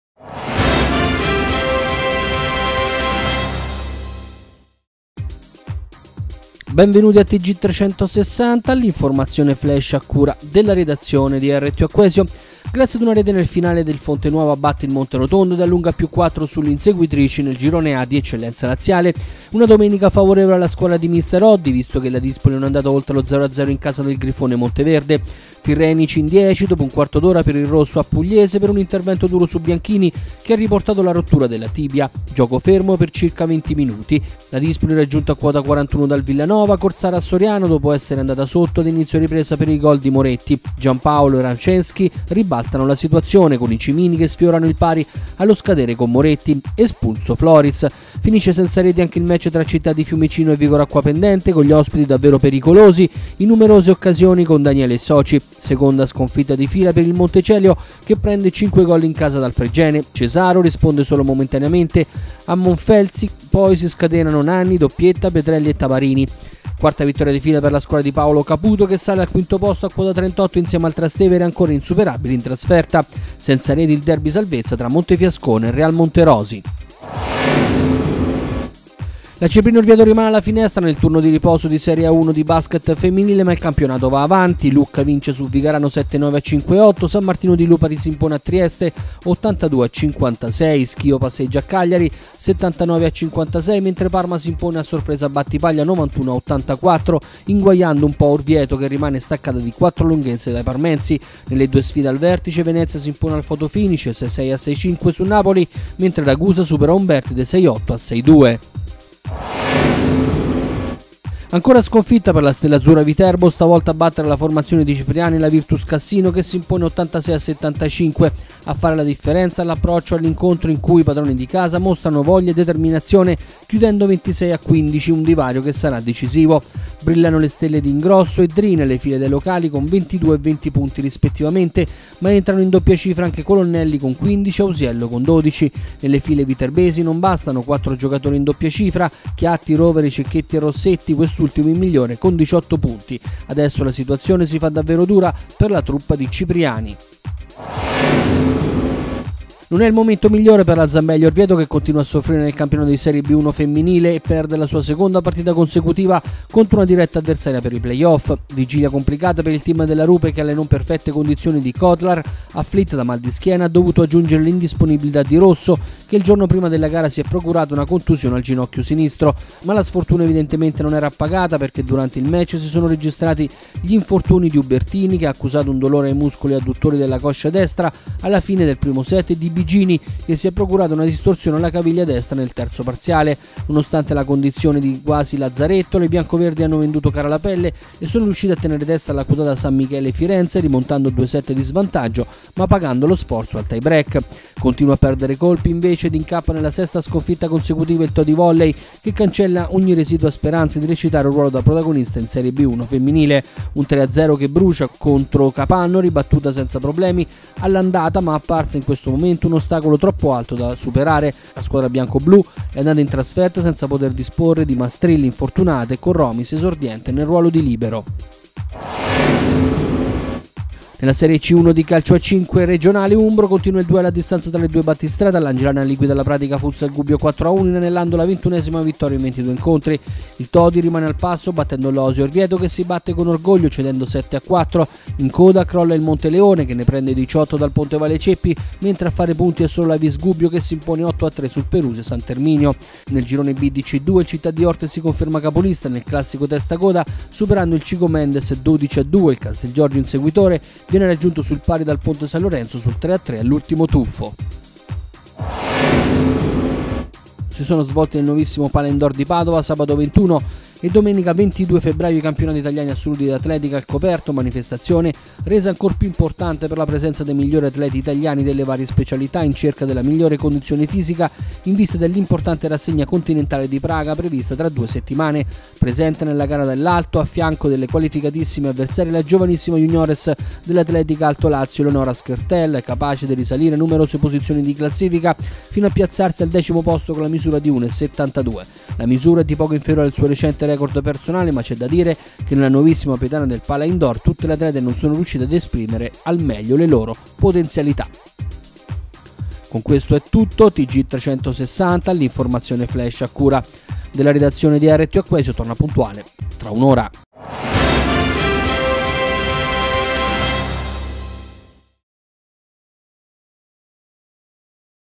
TG360 nr.969
edizione del 25 febbraio 2015
la vigor acquapendente impatta con il fiumicino nell'eccellenza laziale; in A1 turno di riposo per Orvieto, in calendario scontro tra le prime 4 in graduatoria; sconfitta la stella azzurra viterbo a cassino; la zambelli cede in casa contro san michele firenze, il todi volley cede con capannori; il punto sulla serie c1 di calcio a5; ottima prestazione della viterbese schertel agli indoor di padova.